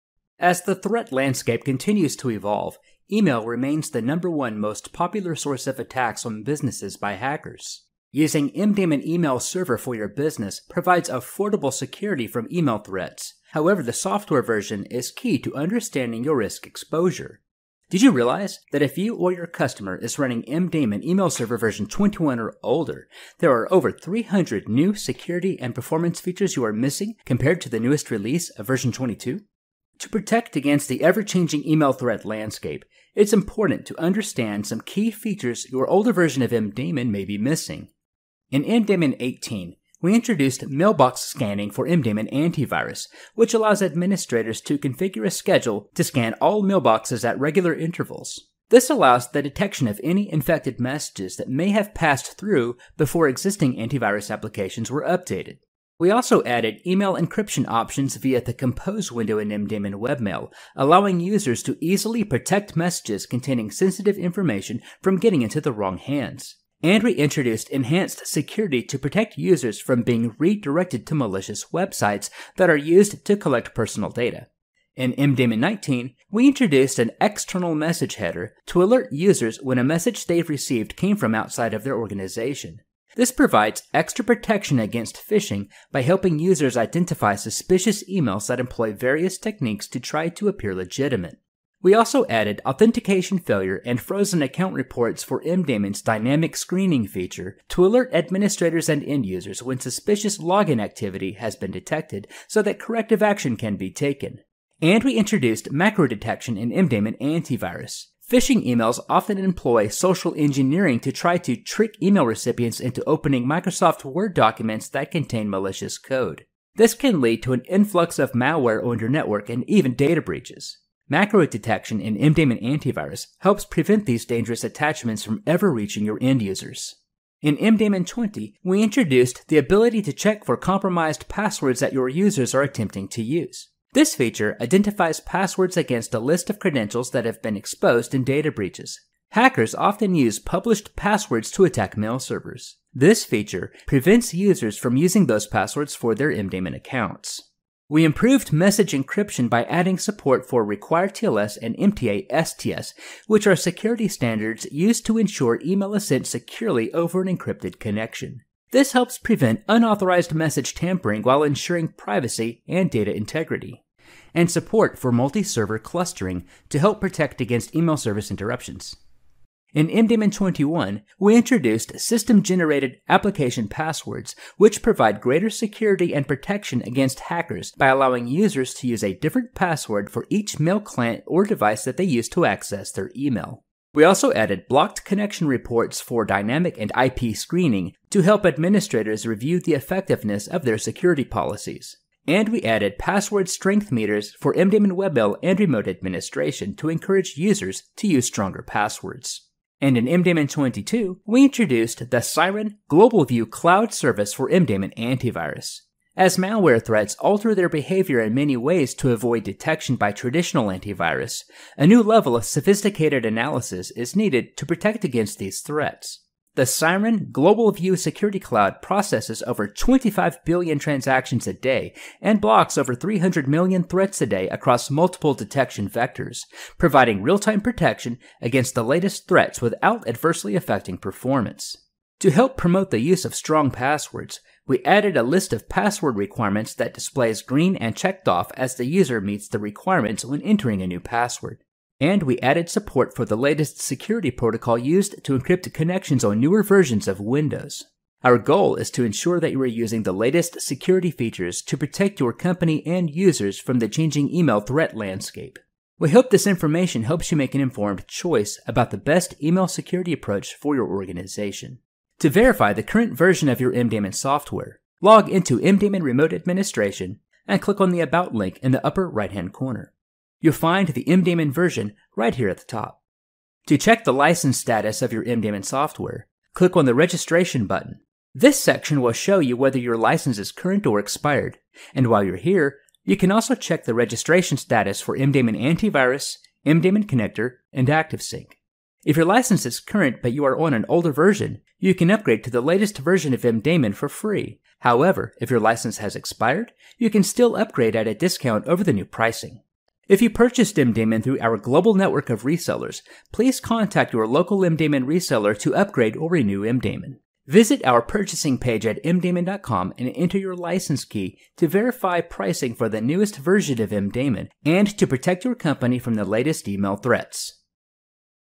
New Security, Collaboration & Administration Features for MDaemon Email Server - Up to Version 22
MDaemon includes over 300 new email security, administration, and collaboration features. In this video, we introduce you to the following features.

In MDaemon 18, we introduced:

Mailbox scanning for MDaemon AntiVirus, which allows administrators to configure a schedule to scan all mailboxes at regular intervals. This allows the detection of any infected messages that may have passed through before existing antivirus applications were updated.

We also added email encryption options via the Compose window in MDaemon Webmail, allowing users to easily protect messages containing sensitive information from getting into the wrong hands.

And we introduced enhanced security to protect users from being redirected to malicious websites that are used to collect personal data. (DNSSEC)

In MDaemon 19, we introduced:

An External Message header – to alert users when a message they’ve received came from outside of their organization. This provides extra protection against phishing by helping users identify suspicious emails that employ various techniques to try to appear legitimate.

We also added authentication Failure and frozen account reports for MDaemon’s Dynamic Screening feature – to alert administrators and end users when suspicious login activity has been detected - so that corrective action can be taken.

And we introduced macro detection in MDaemon AntiVirus.  Phishing emails often employ social engineering to try to trick email recipients into opening Microsoft Word documents that contain malicious code. This can lead to an influx of malware on your network and even data breaches. Macro Detection in MDaemon AntiVirus helps prevent these dangerous attachments from ever reaching your end users.

In MDaemon 20, we introduced:

The ability to check for compromised passwords that your users are attempting to use. This feature identifies passwords against a list of credentials that have been exposed in data breaches. Hackers often use published passwords to attack mail servers. This feature prevents users from using those passwords for their MDaemon accounts.

We improved message encryption by adding support for RequireTLS and MTA-STS, which are security standards used to ensure email is sent securely over an encrypted connection. This helps prevent unauthorized message tampering while ensuring privacy and data integrity.

In MDaemon 21, we introduced:

System-generated application passwords, which provide greater security and protection against hackers by allowing users to use a different password for each mail client or device they use to access their email.

We also added Blocked Connection Reports for Dynamic & IP Screening to help administrators review the effectiveness of their security policies.

And we added password strength meters for MDaemon Webmail and Remote Administration to encourage users to use stronger passwords.

And in MDaemon 22, we introduced:

The Cyren GlobalView™cloud service for MDaemon AntiVirus.

As malware threats alter their behavior in many ways to avoid detection by traditional AV, a new level of sophisticated analysis is needed to protect against these threats. The Cyren GlobalView™ security cloud processes over 25 billion transactions a day and blocks over 300 million threats a day across multiple detection vectors, providing real-time protection against the latest threats without adversely affecting performance.

To help promote the use of strong passwords, we added a list of password requirements that displays green and checked off as the user meets the requirements when entering a new password.

And we added support for the latest security protocol used to encrypt connections on newer versions of Windows.